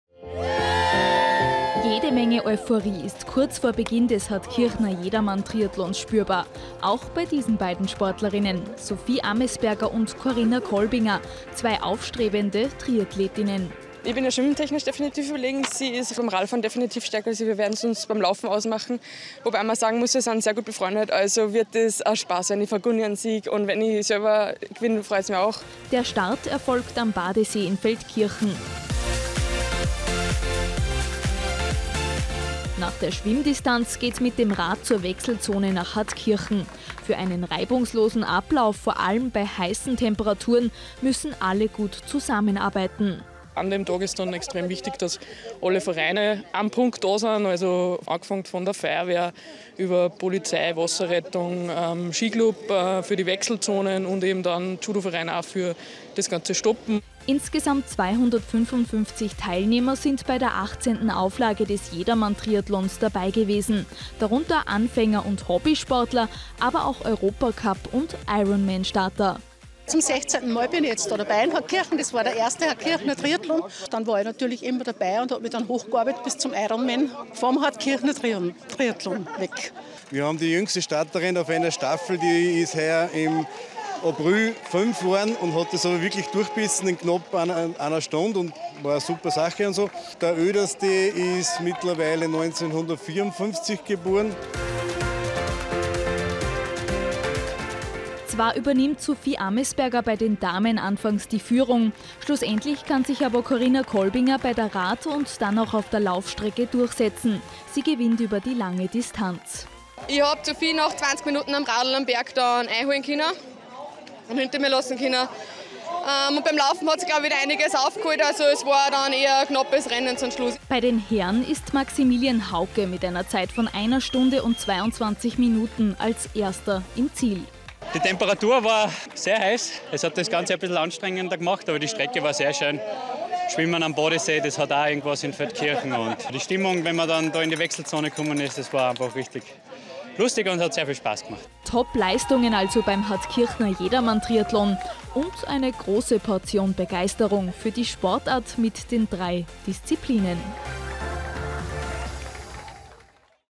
Hartkirchner Jedermann-Triathlon beweist sich als Talent-Schmiede
Drei Sportarten, die es in zwei verschiedenen Bezirken zu absolvieren gilt und Teilnehmer im Alter zwischen fünf und 70 Jahren. Das ist der Hartkirchner Jedermann Triathlon. Der Bewerb hat schon viele Jahre Tradition, einige Triathlon-Asse von heute haben sich dort schon gemessen. So auch heuer wieder.